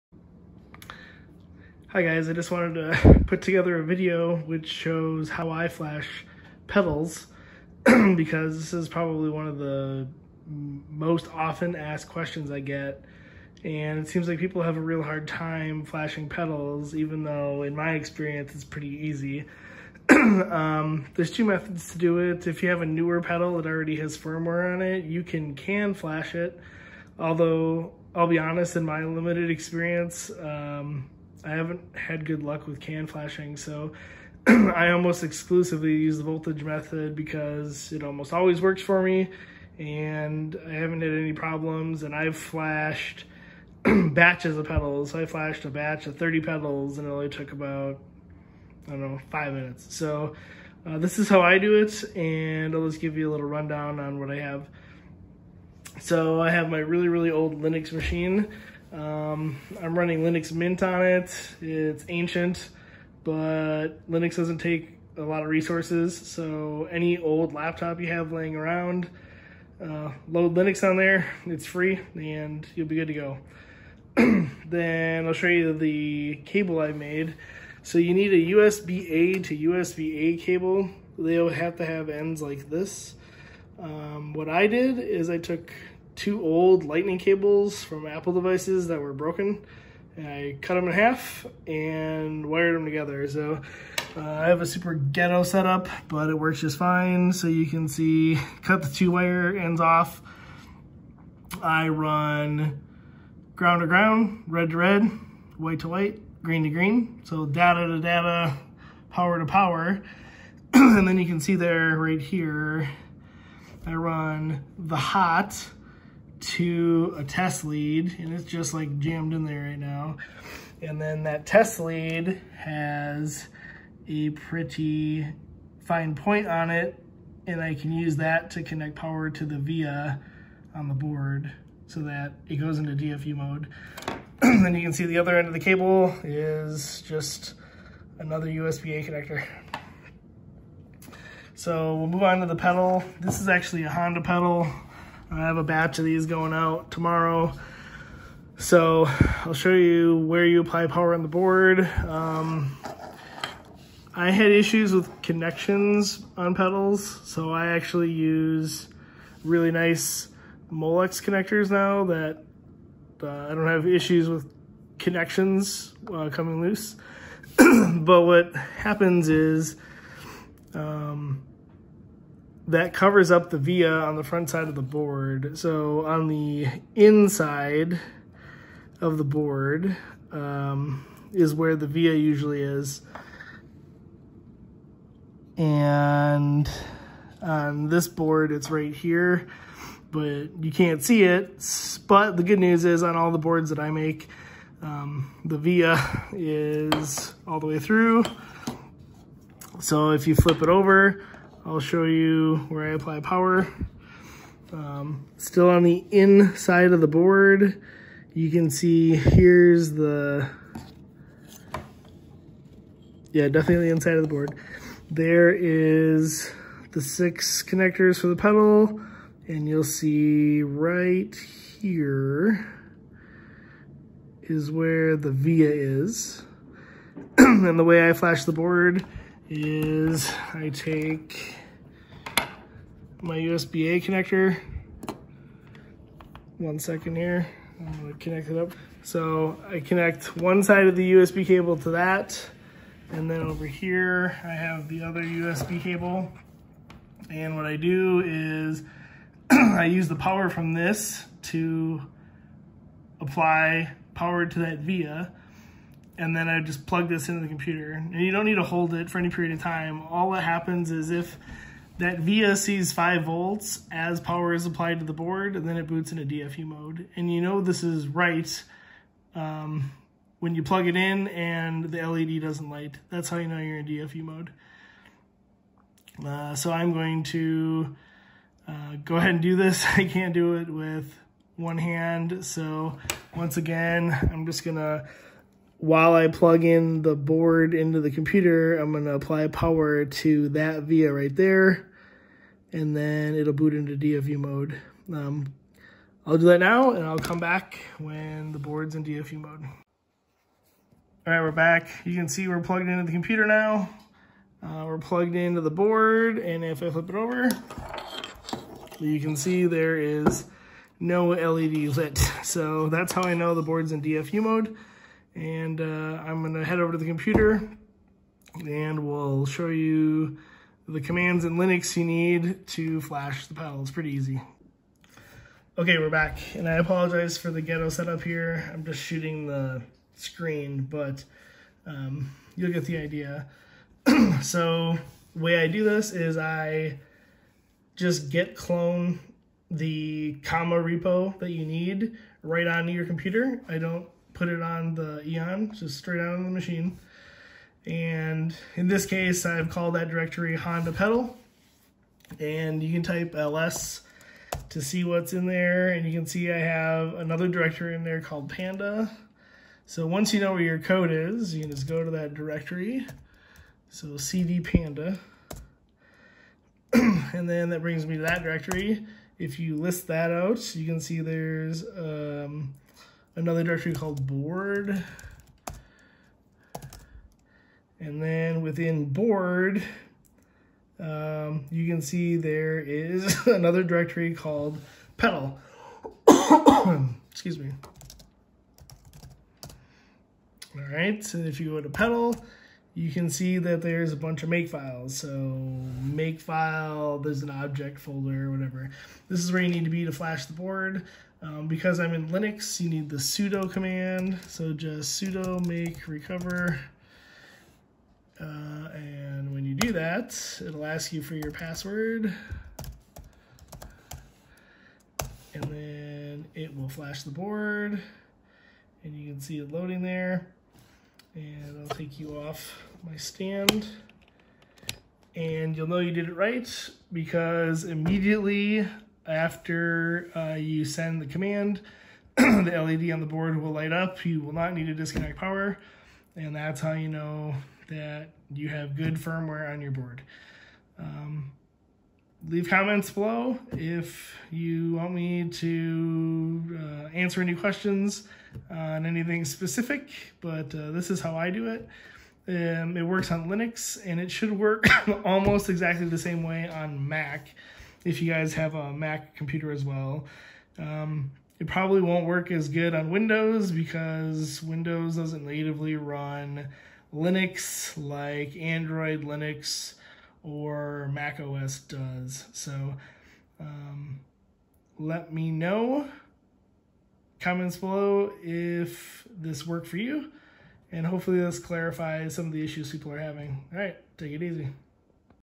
How to Flash a Comma Pedal
Flashing Honda firmware onto a Comma AI pedal interceptor board.  This method will also work with Acura, Toyota, and Tesla vehicles as long as you have the appropriate firmware available for your particular vehicle.

Always remain aware of your surroundings and in control of your vehicle when using any level 2 self driving equipment.
This device is not for on-road use...use at your own risk.

Link to Level 2 Tesla ebay store: